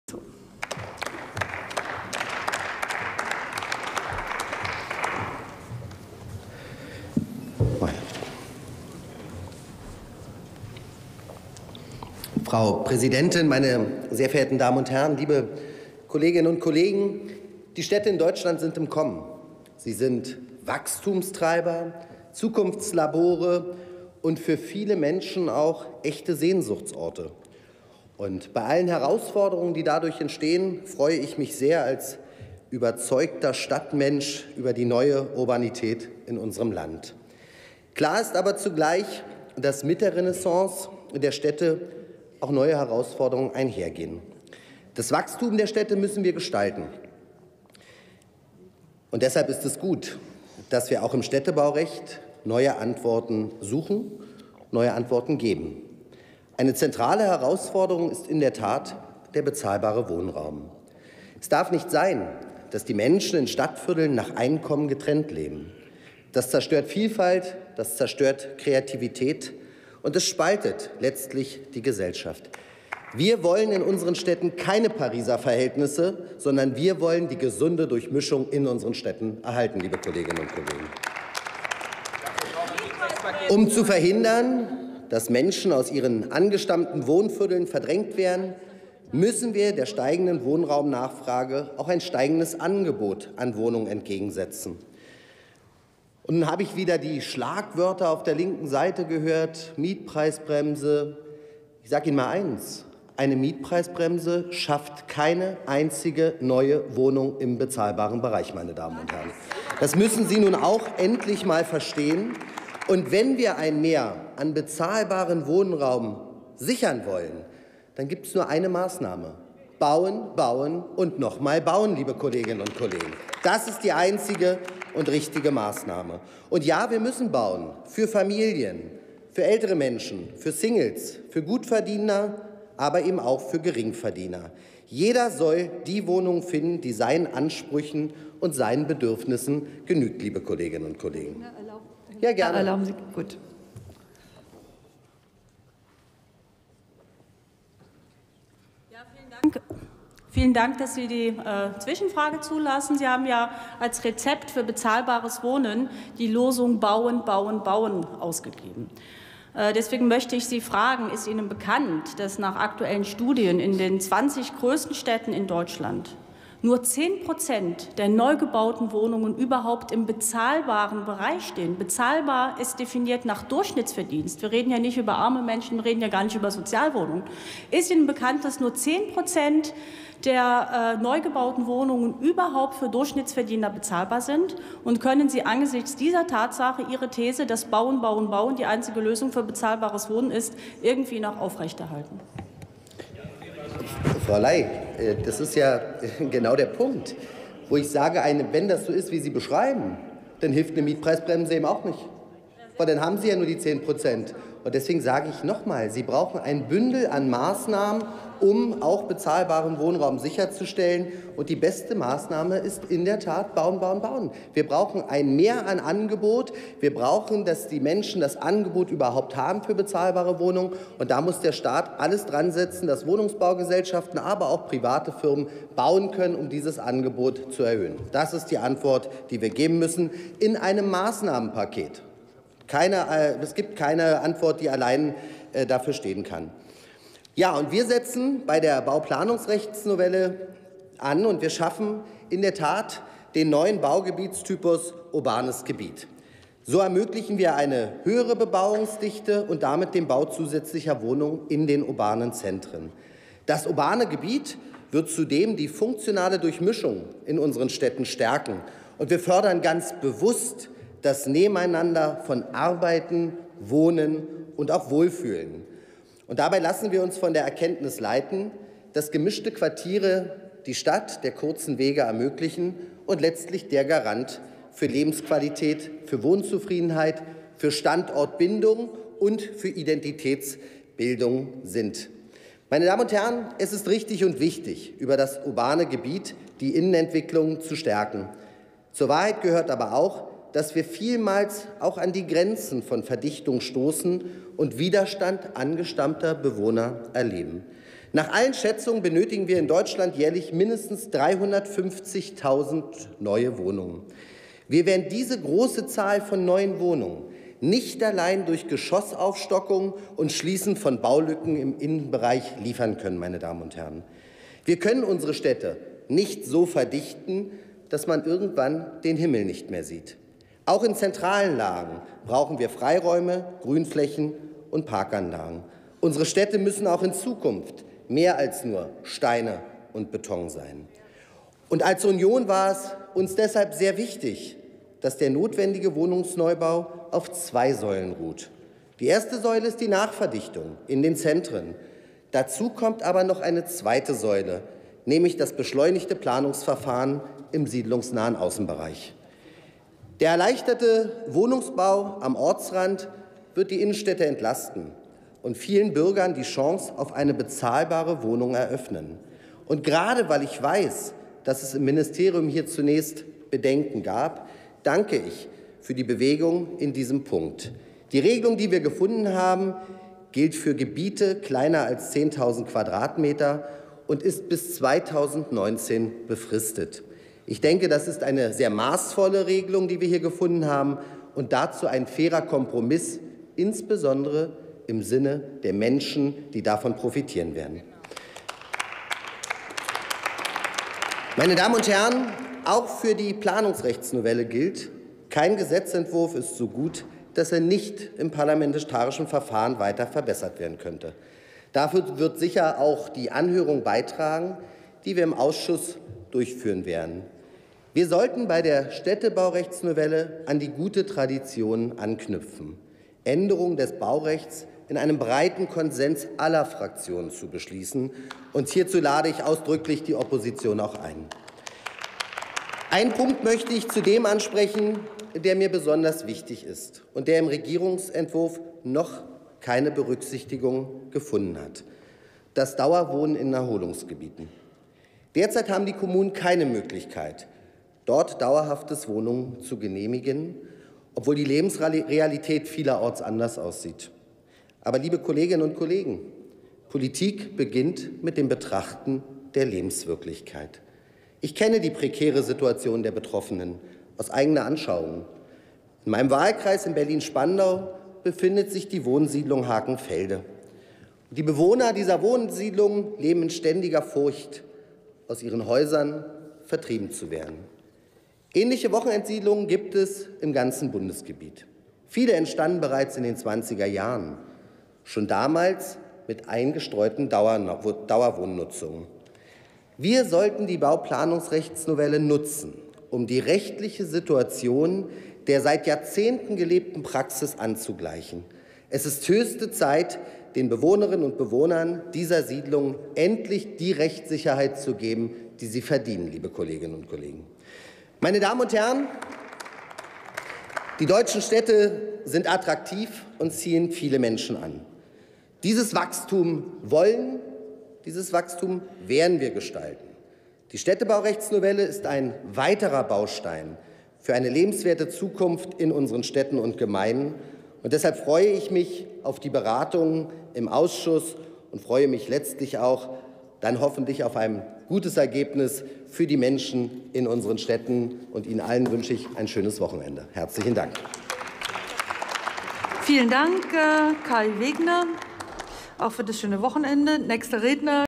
Kai Wegner: Umsetzung einer EU-Richtlinie im Städtebaurecht [Bundestag 27.01.2017]
eingebrachten Entwurfs eines Gesetzes zur Umsetzung der Richtlinie 2014/52/EU im Städtebaurecht und zur Stärkung des neuen Zusammenlebens in der Stadt
Drucksache 18/10942

Bemerkungen
13:15:40 Zwischenfrage: Lay, Caren (Die Linke)

18. Deutscher Bundestag
216. Plenarsitzung
Tagesordnungspunkt: Umsetzung einer EU-Richtlinie im Städtebaurecht
Redner: Wegner, Kai, CDU/CSU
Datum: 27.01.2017
Plenarprotokoll:
UVID:7064838